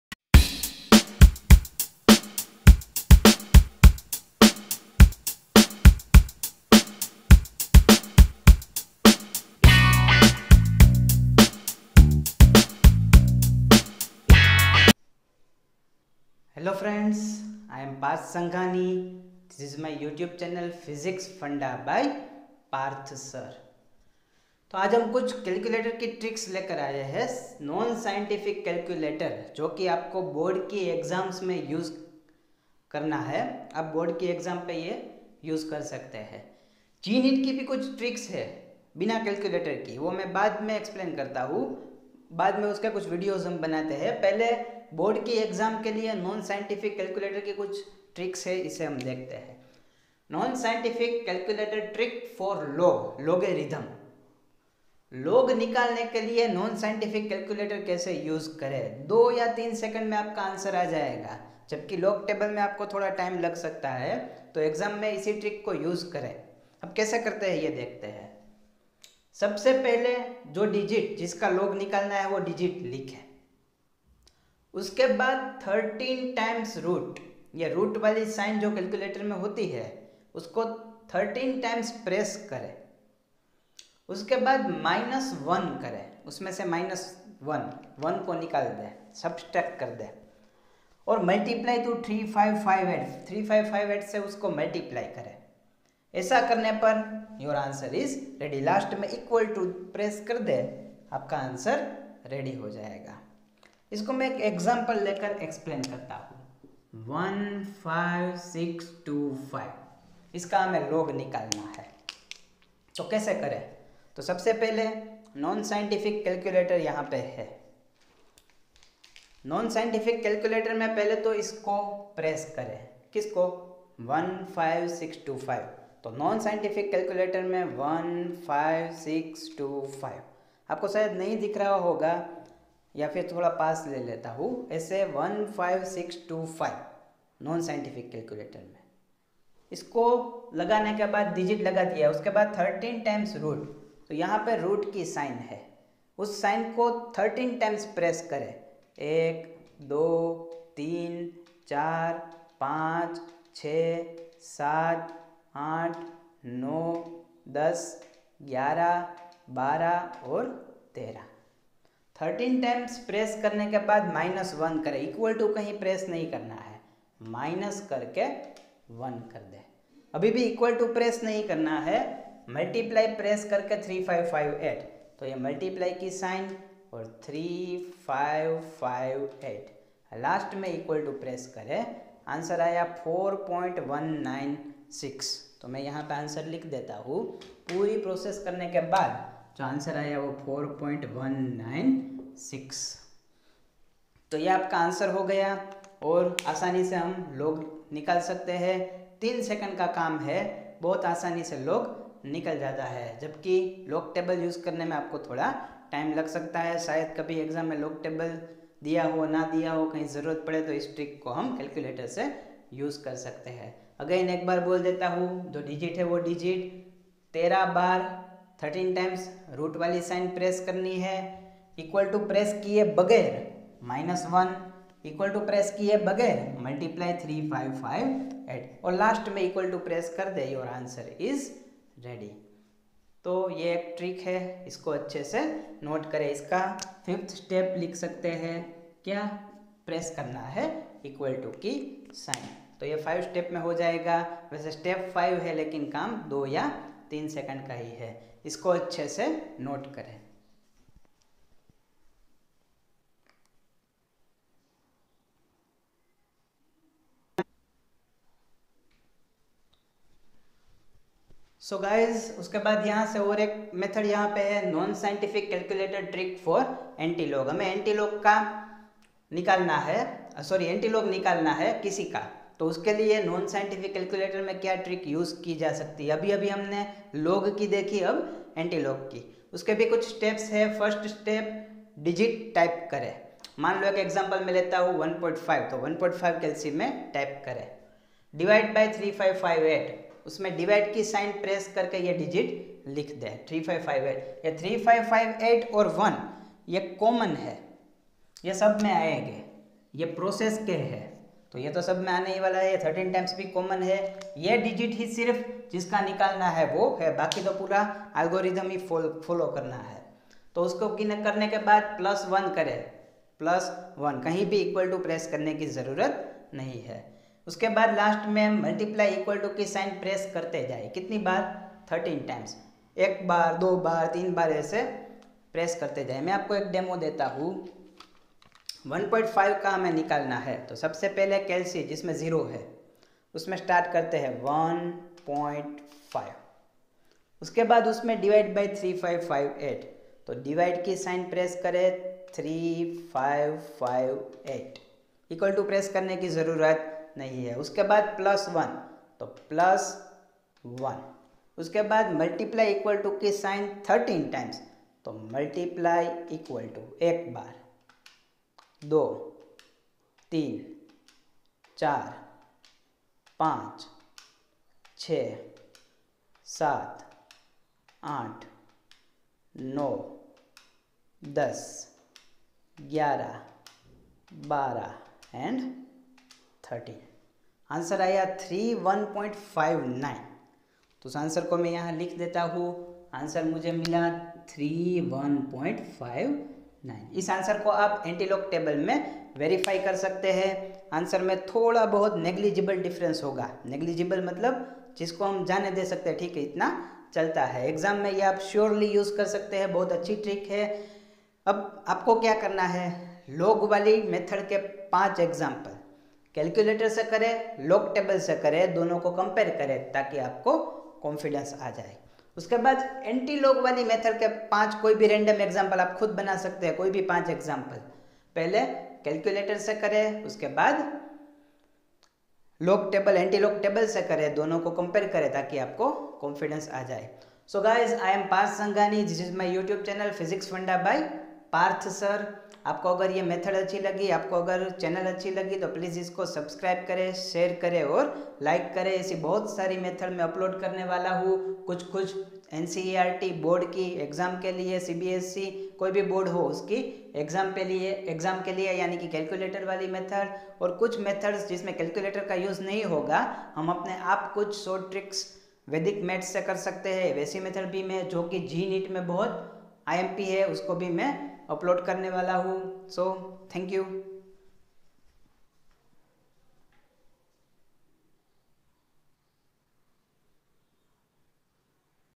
Tricks for log and Antilog
This video describes how to use nonscientific calculator to find logarithm and antilogarithm.